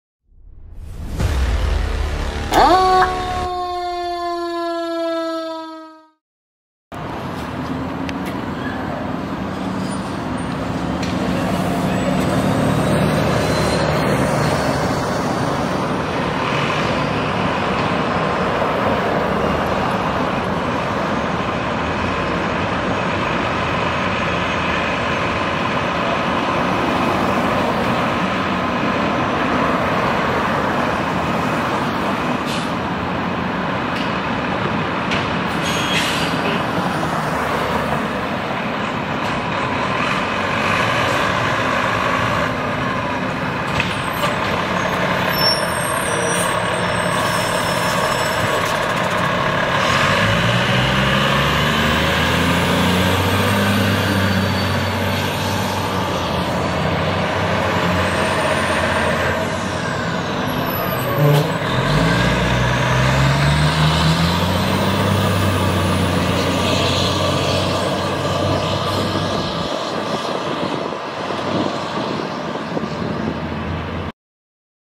Laval Fire Department (SSIL) Spare Pumper 258 & Spare Ladder 459 responding from fire station 9.
Laval Fire Spare Pumper 258 (209) & Spare Ladder 459 (409, EX-409) responding to an emergency call from fire station 9 in the Vimont district of Laval.

-Footage taken: April 11th, 2023

🎥-Video Informations:

🔨Equipment: Canon EOS 250D

💻Editing application: CapCut (Mobile)
Thumbnail: Adobe EXPRESS